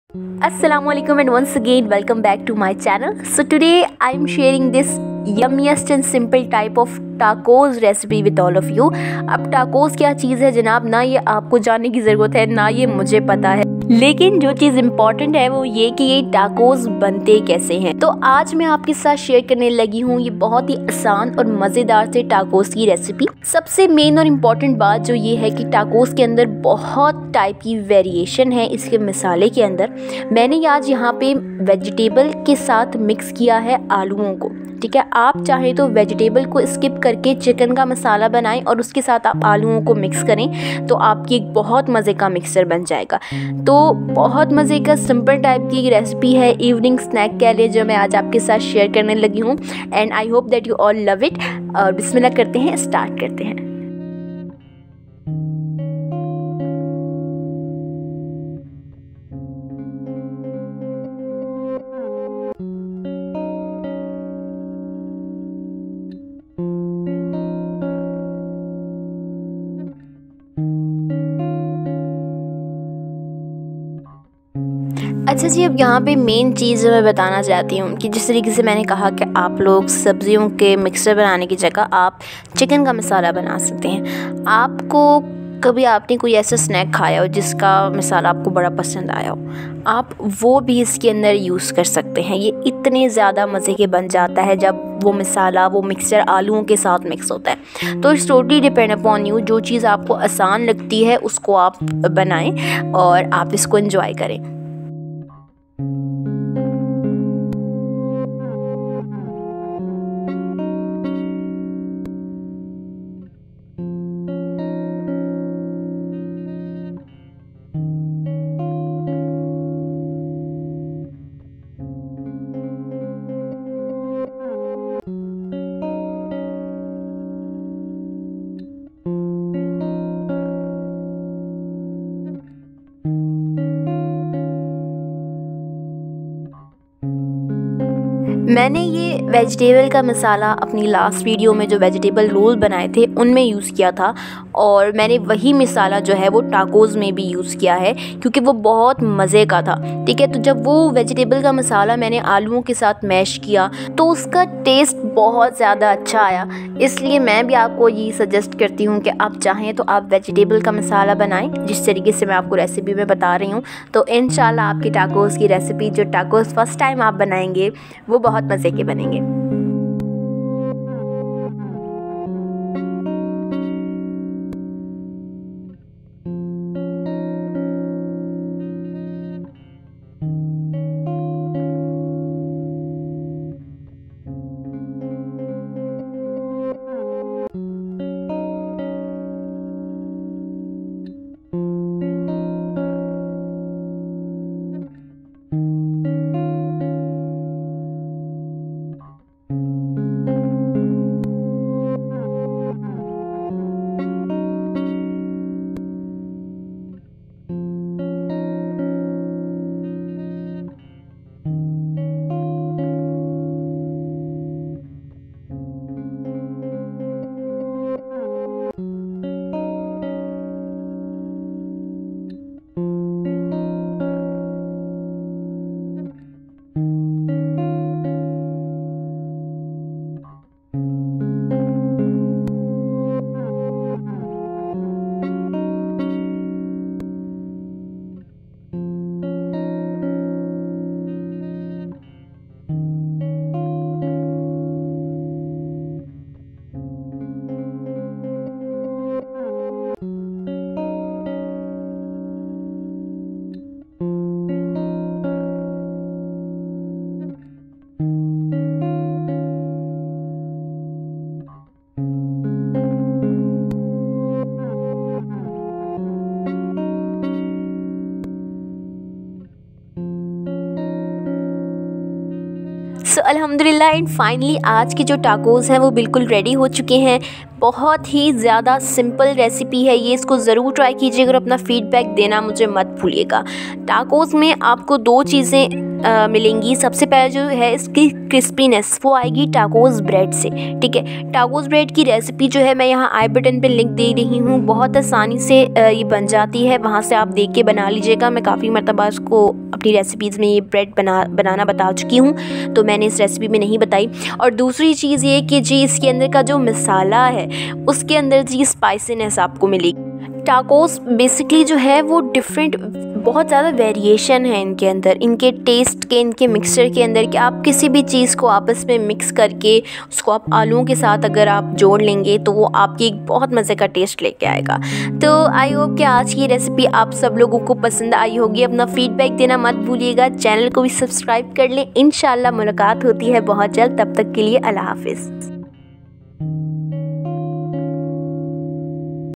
Spicy Mexican Tacos by Mahnoor jawaid ❤️
Simple evening snack with vegetable that you all love 🌸